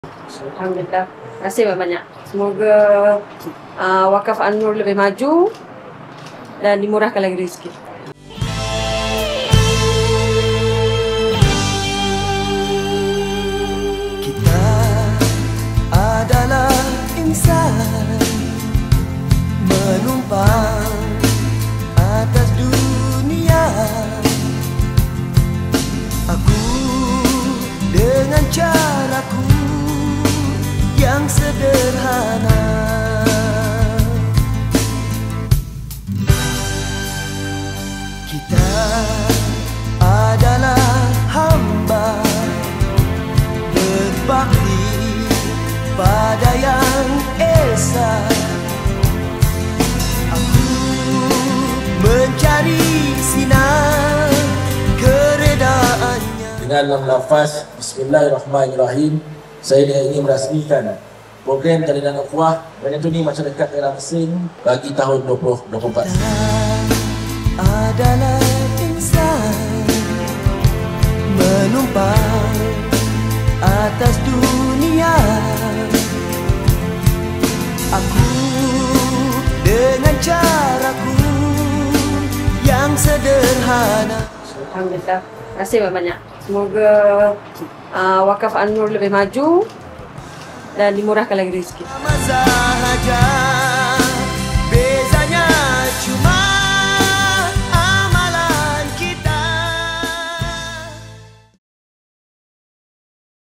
PROGRAM JALINAN UKHUWAH MERSING
Pada 10 November lalu, Waqaf An-Nur  bersama Pejabat Daerah dan Pejabat Kadi Mersing menganjurkan Program Jalinan Ukhuwah (JU) di Masjid Sungai Mengkuang, Teriang Kecil, Endau.

Program JU bertujuan mendekatkan Waqaf An-Nur dengan masyarakat serta memperkukuh hubungan komuniti, termasuk golongan M40, B40, mualaf, gelandangan, dan orang asli.

Menurut Ketua Divisyen Pembangunan Ummah Waqaf An-Nur, Tn. Hj. Zamri Norani, program ini merupakan salah satu inisiatif untuk menyalurkan manfaat wakaf yang diuruskan Waqaf An-Nur, sekali gus membuktikan manfaat wakaf adalah untuk semua tanpa mengira bangsa dan agama.

Antara aktiviti yang dilaksanakan ialah penyerahan dua rumah asnaf bernilai RM164 ribu, gotong-royong, pemeriksaan kesihatan percuma, serta bantuan bakul makanan bernilai RM10,000. Pelajar terpilih turut menerima sumbangan "Back to School" bernilai RM17,500, sementara Masjid Sungai Mengkuang menerima RM1,000.

Program dirasmikan oleh Tuan Haji Jamil Hasni Abdullah, Pegawai Daerah Mersing, yang menyokong penuh usaha memperkukuh hubungan antara masyarakat dan institusi wakaf di Johor.